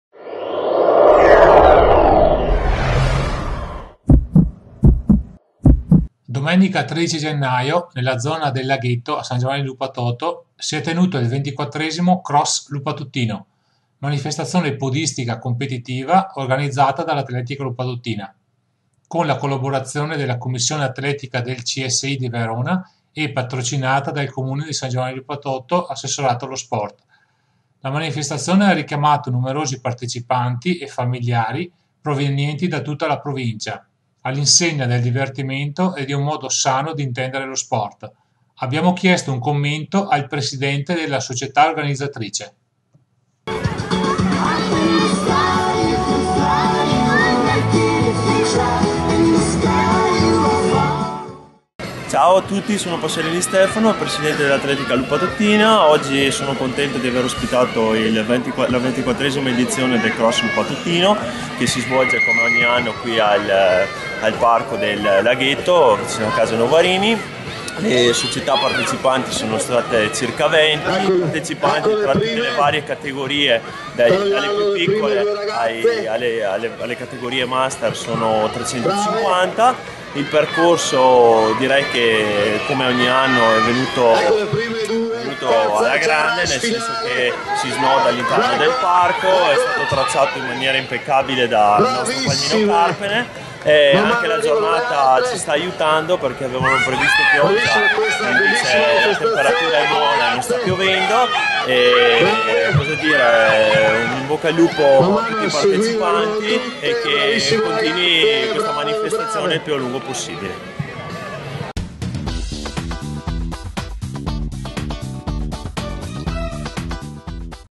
San Giovanni Lup: invasione di podisti per il 24' Cross lupatotino
Domenica 13 gennaio si è svolto a San Giovanni Lupatoto la 24^ edizione del cross lupatotino, manifestazione inserita nel calendario provinciale di corsa campestre. Come si vede dalle immagini girate nella zono Laghetto nei pressi di Casa Novarini, la partecipazione di ragazzi e amatori è stata molto numerosa circa 350 atleti; se a questi aggiungiamo i genitori e allenatori dei piccoli atleti, la zona del parco era invasa da 600/700 persone, niente male per una domenica mattina invernale. Si sono susseguite le partenze delle varie categorie, dai cuccioli, agli esordienti di cui vediamo le immagini e a seguire tutte le altre categorie fino agli amatori. Iscrizioni gratuite per i ragazzi, in omaggio una barretta di cioccolato a tutti e premi a parte ai primi 5 arrivati di ogni categoria. L'organizzazione è stata curata in tutti i minimi dettagli dal gruppo podistico Atletica Lupatotina che ha ricevuto numerosi consensi da parte dei presenti. L'augurio è che questa bella manifestazione possa continuare anche in futuro.